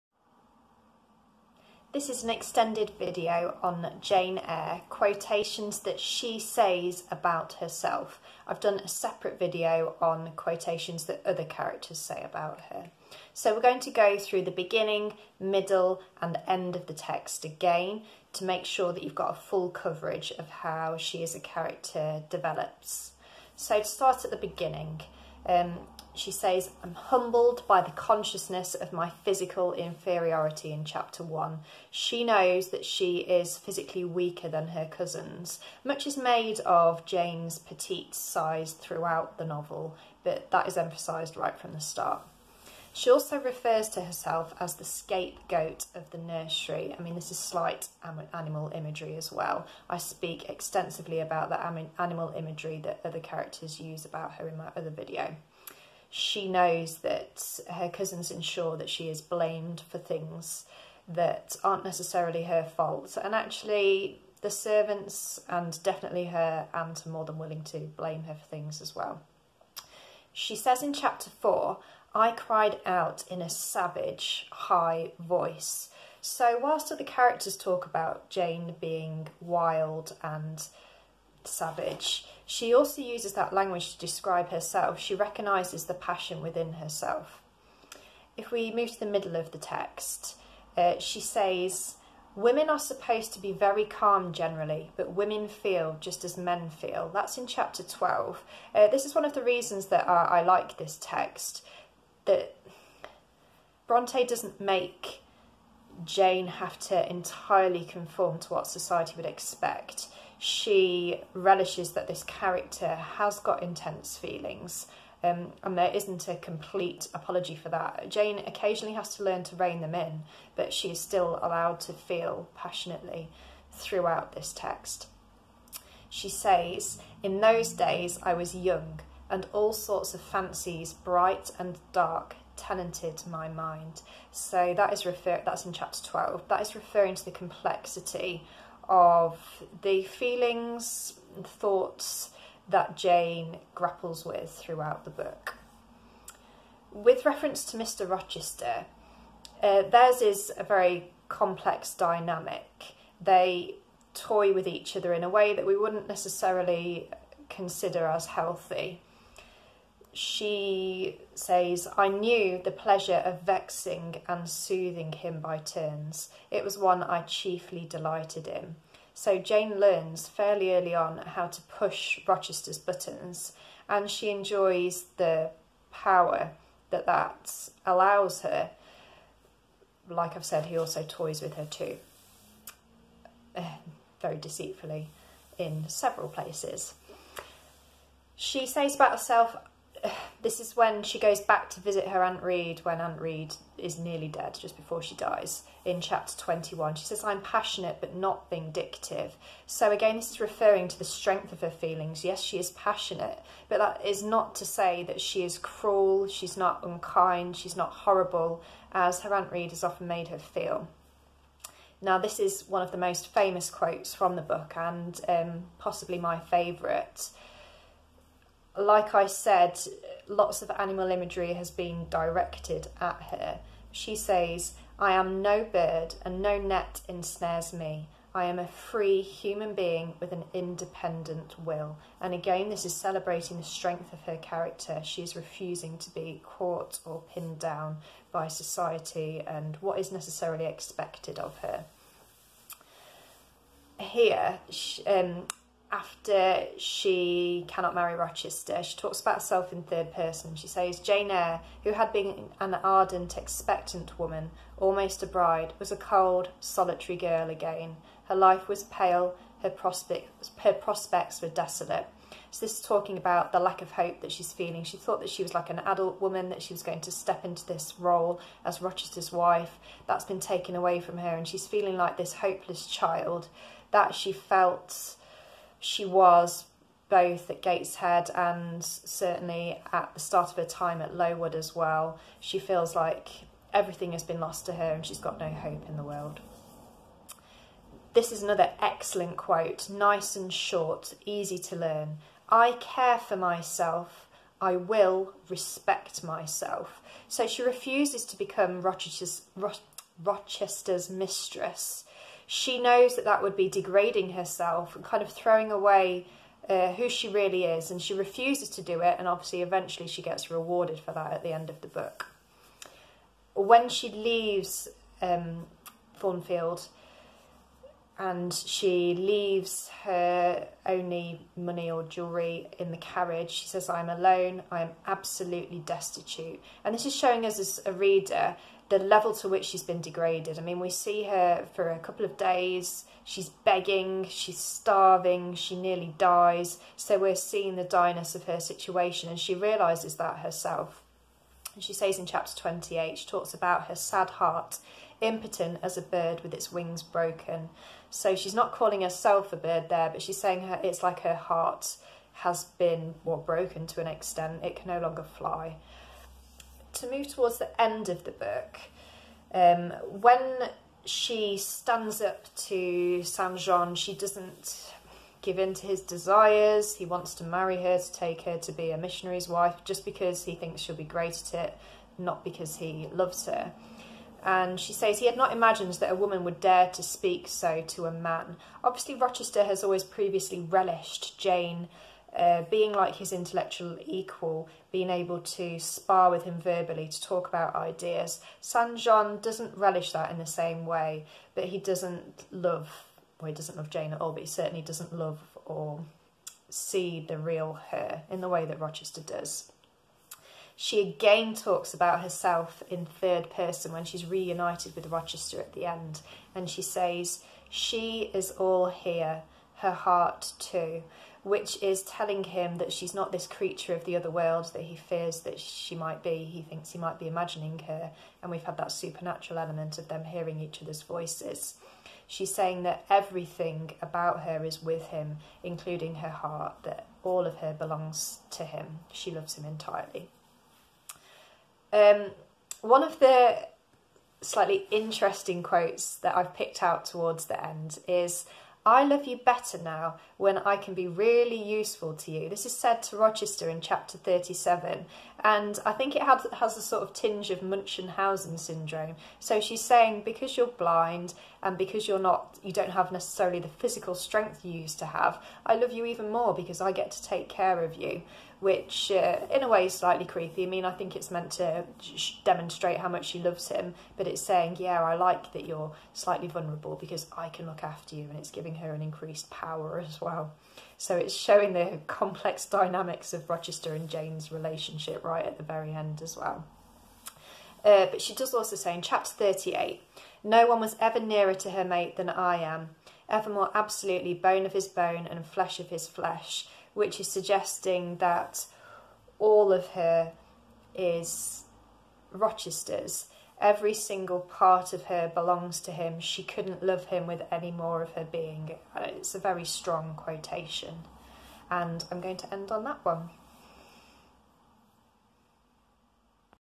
Jane Eyre - Quotes by Herself - Extended (GCSE Revision)
An extended look at quotations about the character of Jane Eyre said by herself.

These cover the beginning, middle and end of the book.